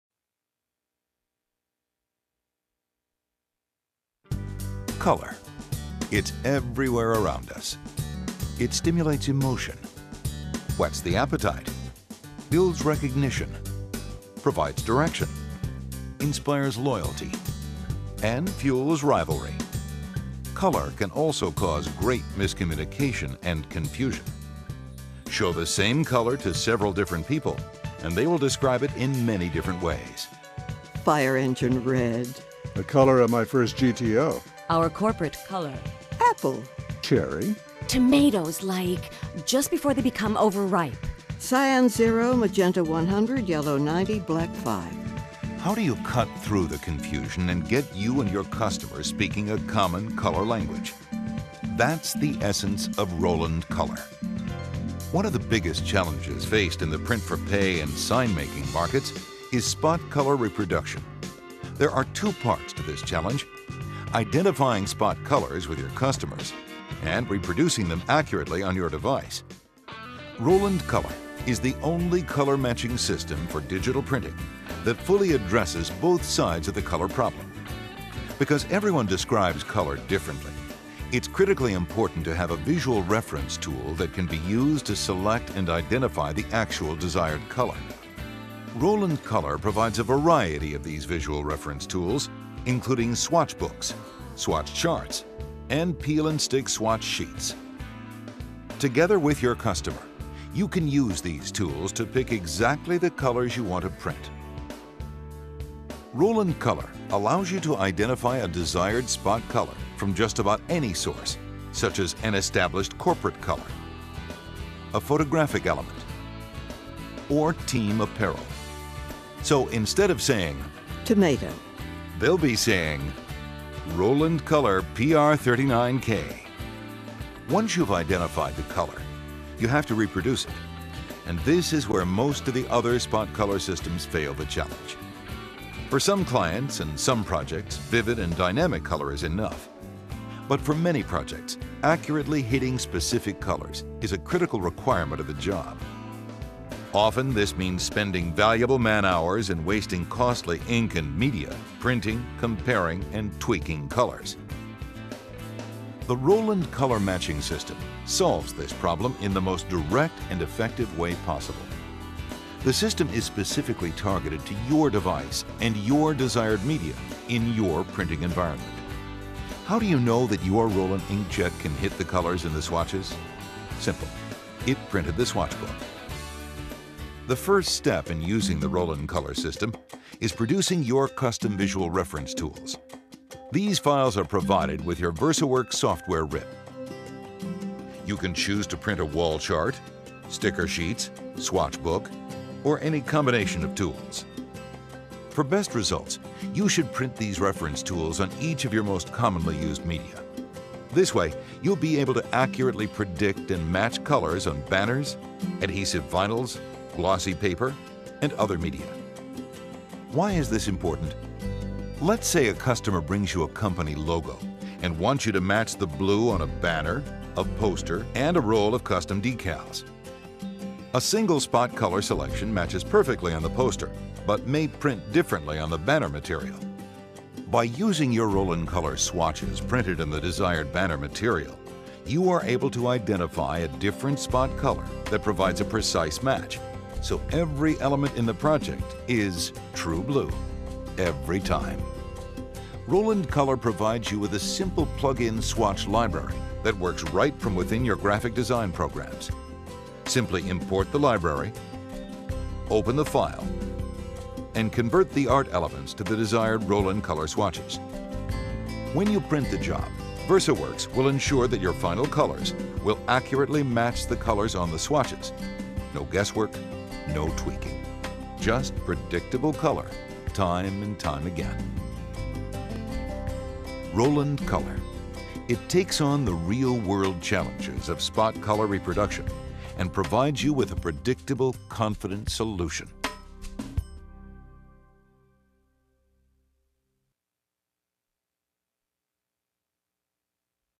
Advanced Color Solutions - Spot Color Matching Made Easy
Learn why Roland Color is the only spot color matching system that allows you to digitally print spot colors accurately every time with no guess work, no tweaking and no wasted prints.  It's not a target... it's a promise.

Roland Color is a feature included in VersaWorks RIP, developed by Roland engineers exclusively for the Roland inkjet line.